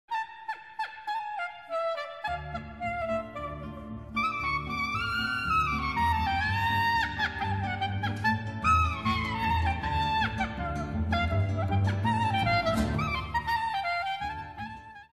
Larval mask in rehearsal
Rehearsing for The Audition - Johanna carries larval mask 'Beko' by TheatrElan